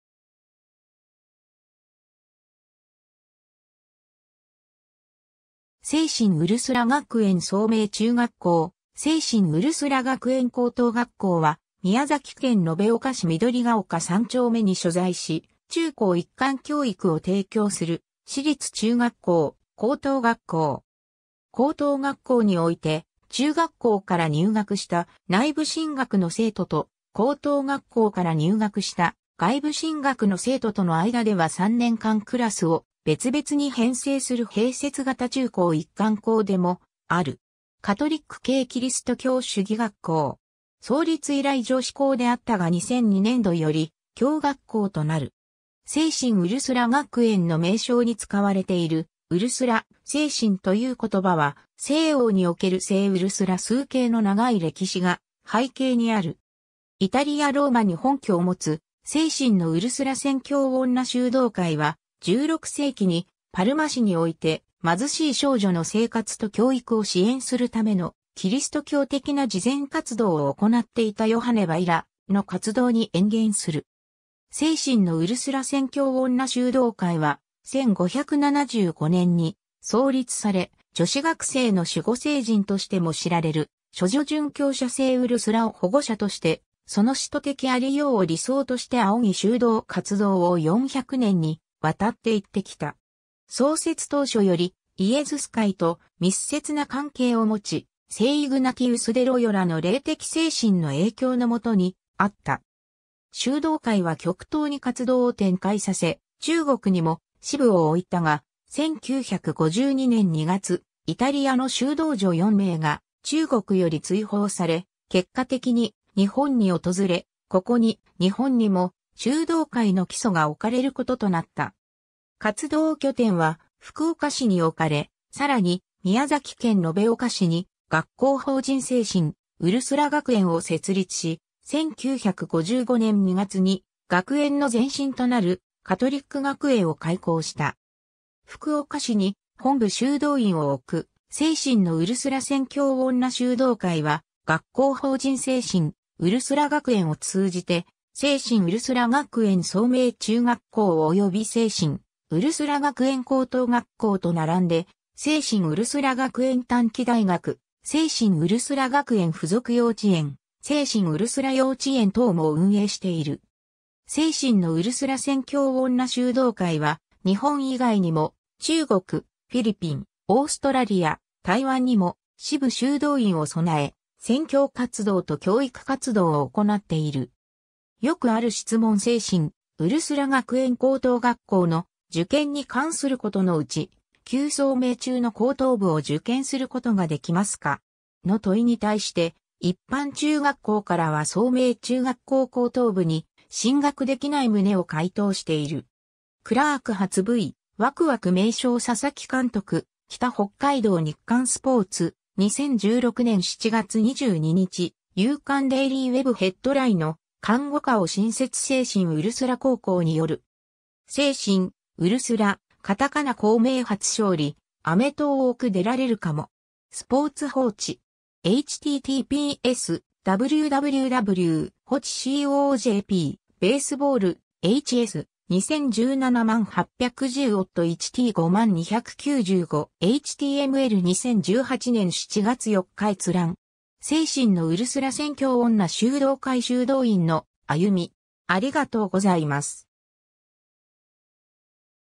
聖心ウルスラ学園聡明中学校・聖心ウルスラ学園高等学校
聖心ウルスラ学園聡明中学校・聖心ウルスラ学園高等学校（せいしんウルスラがくえんそうめいちゅうがっこう・せいしんウルスラがくえんこうとうがっこう）は、宮崎県延岡市緑ヶ丘三丁目に所在し、中高一貫教育を提供する私立中学校・高等学校。
 高等学校において、中学校から入学した内部進学の生徒と、高等学校から入学した外部進学の生徒との間では3年間クラスを別々に編成する併設型中高一貫校でもある。
カトリック系キリスト教主義学校（ミッション・スクール）。
創立以来女子校であったが2002年度より、共学校となる。
 聖心ウルスラ学園の名称に使われている「ウルスラ」・「聖心」という言葉は、西欧における聖ウルスラ崇敬の長い歴史が背景にある。
 イタリア・ローマに本拠を持つ「聖心のウルスラ宣教女修道会（Orsoline Missionarie del Sacro Cuore、みこころのうるすらせんきょうじょしゅうどうかい）」は、16世紀に、パルマ市において、貧しい少女の生活と教育を支援するためのキリスト教的な慈善活動を行っていたヨハネ・ヴァイラの活動に淵源する。
聖心のウルスラ宣教女修道会は、1575年に創立され、女子学生の守護聖人としても知られる処女殉教者聖ウルスラを保護者として、その使徒的ありようを理想として仰ぎ修道活動を400年に渡って行ってきた。
 創設当初よりイエズス会と密接な関係を持ち、聖イグナティウス・デ・ロヨラの霊的精神の影響の元にあった。
修道会は極東に活動を展開させ、中国にも支部を置いたが、1952年2月、イタリアの修道女4名が中国より追放され、結果的に日本に訪れ、ここに日本にも修道会の基礎が置かれることとなった。
活動拠点は福岡市に置かれ、更に宮崎県延岡市に「学校法人聖心ウルスラ学園」を設立し、1955年2月に学園の前身となるカトリック学園を開校した。
 福岡市に本部修道院を置く「聖心のウルスラ宣教女修道会（日本）」は、「学校法人聖心ウルスラ学園」を通じて、聖心ウルスラ学園聡明中学校及び聖心ウルスラ学園高等学校と並んで、「聖心ウルスラ学園短期大学」、「聖心ウルスラ学園付属幼稚園」、「聖心ウルスラ幼稚園」等も運営している。
 「聖心のウルスラ宣教女修道会」は、日本以外にも、中国、フィリピン、オーストラリア、台湾にも支部修道院を備え、宣教活動と教育活動を行っている。
 ^ よくある質問－聖心ウルスラ学園高等学校の「受験に関すること」のうち、「Q 聡明中の高等部を受験することができますか。」の問に対して、一般中学校からは聡明中学校高等部に進学できない旨を回答している。
 ^ クラーク初Ｖ「ワクワク」名将佐々木監督／北北海道 - 日刊スポーツ、2016年7月22日 ^ 夕刊デイリーWebヘッドライ（2012年7月12日掲載）の「看護科を新設－聖心ウルスラ高校」による。
^ “聖心ウルスラ、“カタカナ校名”初勝利　「アメトーーク」出られるかも…” (日本語). スポーツ報知. (2017年8月11日). 2018年7月4日閲覧。
 ^ 聖心のウルスラ宣教女修道会 修道院のあゆみ